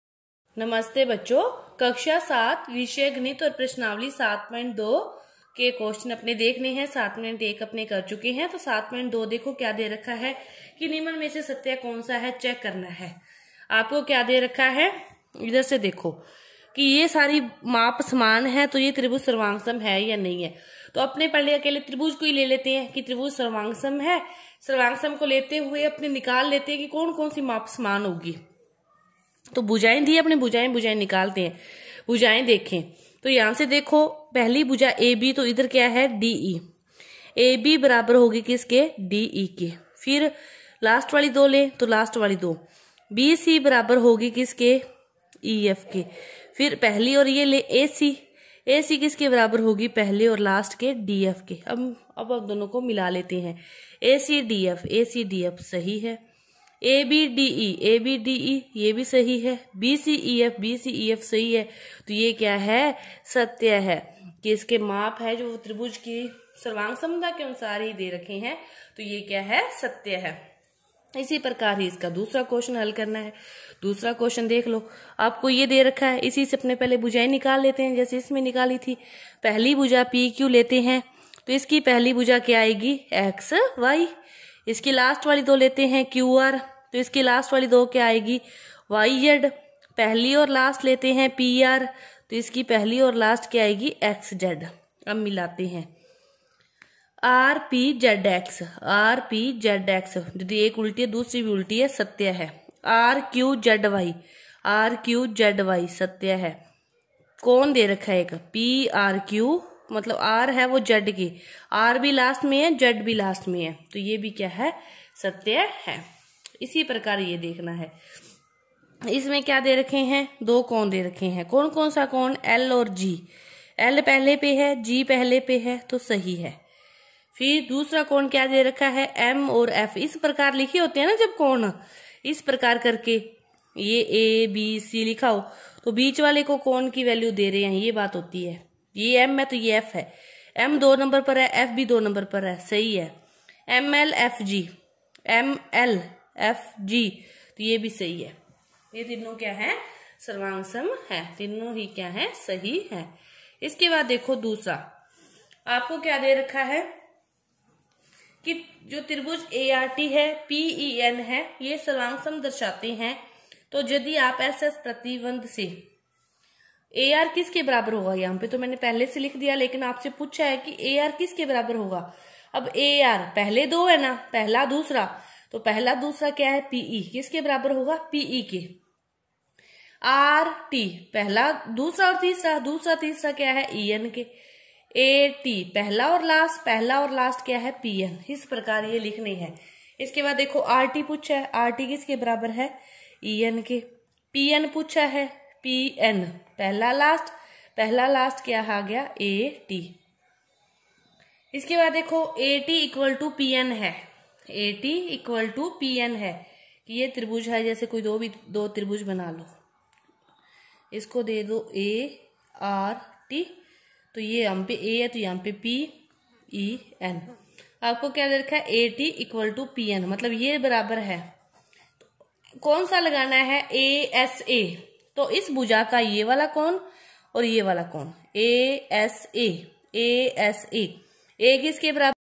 CLASS 7 MATH exercise 7.2 PART 1 कक्षा 7 गणित प्रश्‍नावली 7.2 PART 1 त्रिभुजों की सर्वांगसमता
CLASS 7 MATH exercise 7.2 PART 1 कक्षा 7 गणित प्रश्‍नावली 7.2 PART 1 त्रिभुजों की सर्वांगसमता का ऑनलाइन अध्ययन